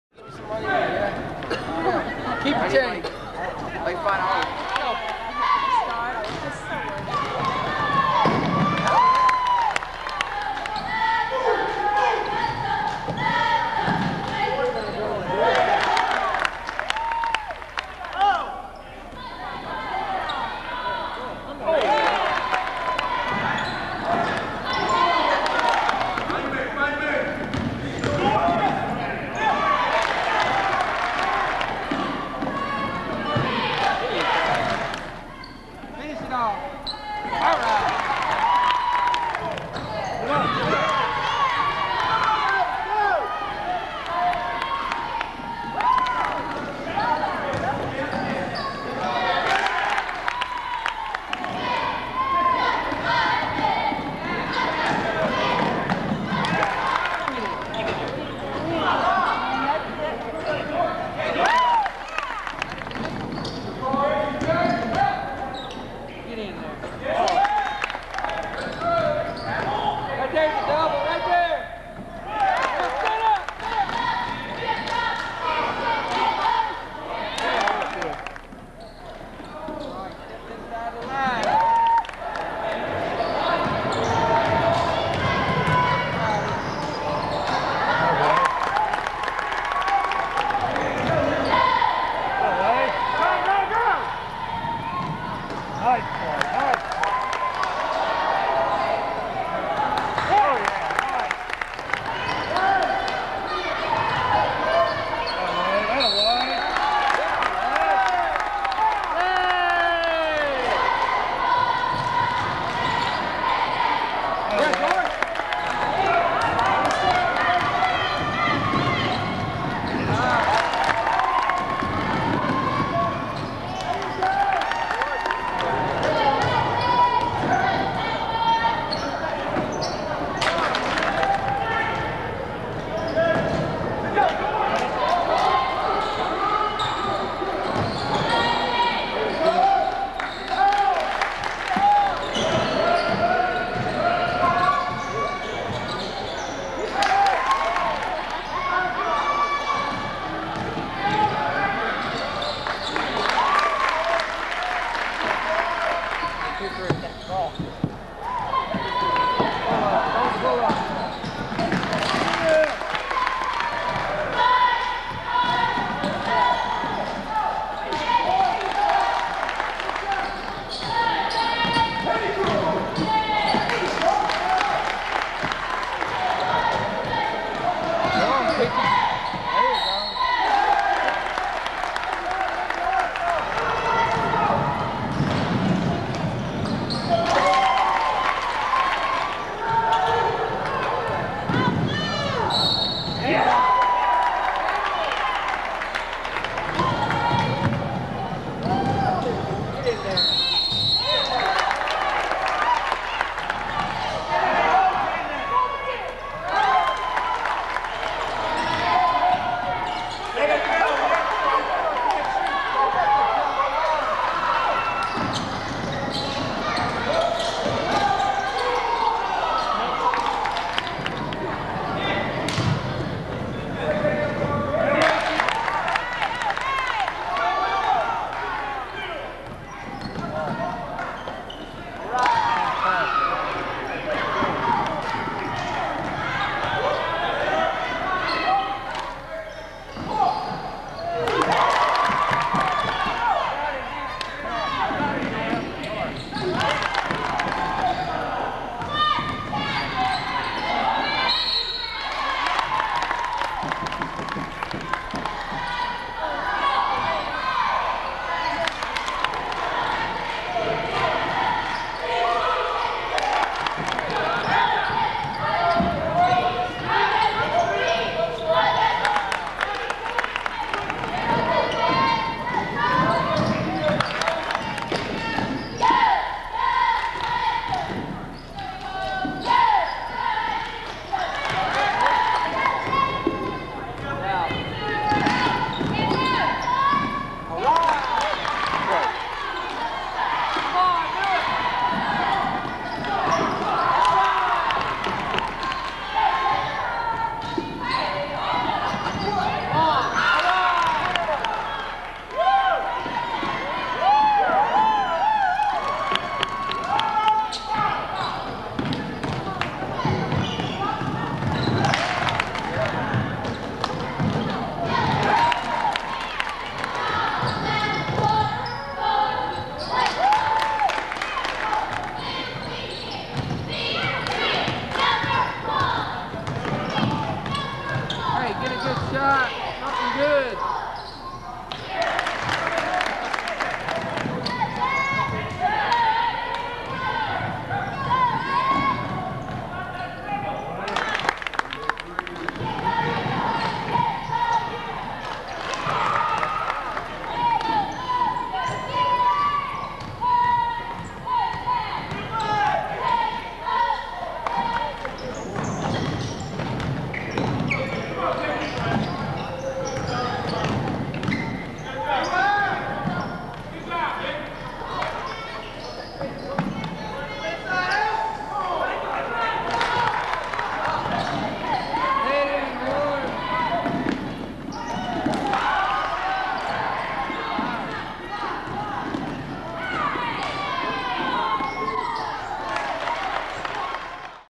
George Goode: Athens Drive JV basketball highlights
From the 1997-98 season